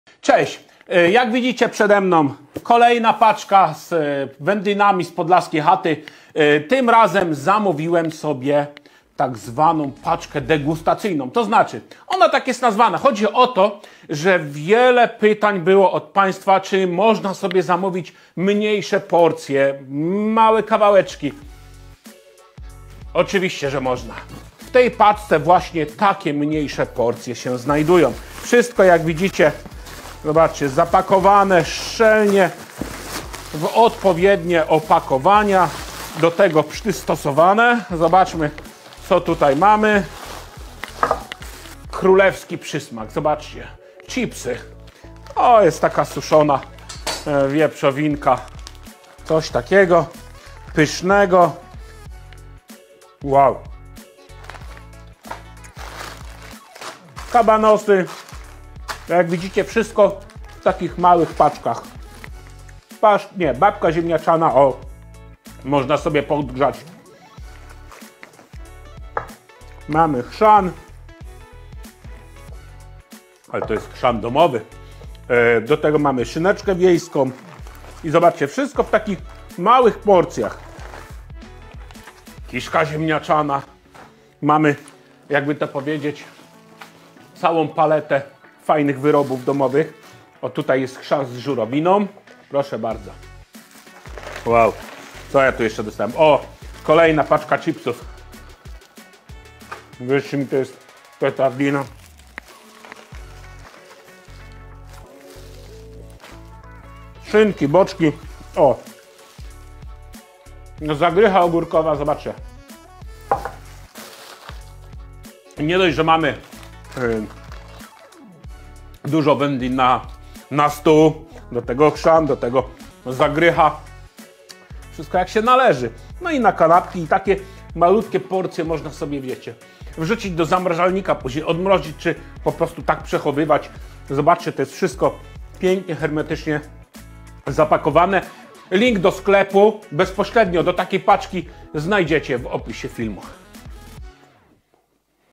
PACZKA Wędlin - Tomasz Strzelczyk ODDASZFARTUCHA POLECA ! Swojskie wyroby wędliniarskie z Podlasia
**Odkryj smaki tradycji z Tomaszem Strzelczykiem! 🍖🌿**

W swoim najnowszym video, OddaszFartucha - Tomasz Strzelczyk, zabiera nas w podróż do korzeni prawdziwej, swojskiej kuchni. Prezentuje on paczkę degustacyjną z 14 różnymi, naturalnymi wyrobami, które są kwintesencją tradycyjnego smaku, jak za dawnych lat - bez żadnych ulepszaczy!

🍂 Każdy produkt w tej paczce to hołd dla dawnych metod wędzenia, peklowania i suszenia, przypominający o bogactwie prostoty i naturalności.

Zachęcamy do obejrzenia video, gdzie Tomasz dzieli się swoimi doświadczeniami i przemyśleniami na temat każdego z tych unikatowych wyrobów.

Dołącz do nas w tej smakowitej podróży do przeszłości! 🌾🔥